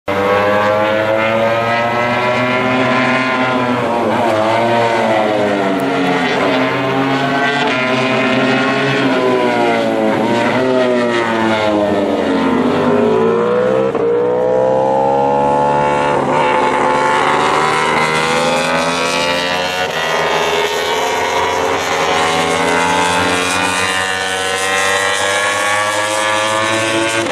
2017motoGP 2人乗りDucati X2
motoGPｲﾍﾞﾝﾄ　前半ﾗｲﾀﾞｰ : ﾗﾝﾃﾞｨｰﾏﾓﾗ、後半ﾗｲﾀﾞｰ : ﾌﾗﾝｺﾊﾞｯﾀｲｰﾆ
後ろに素人乗せﾍﾋﾞｰｳｴｯﾄのﾓﾃｷﾞ4ｺｰﾅｰをｳｨﾘｰ‼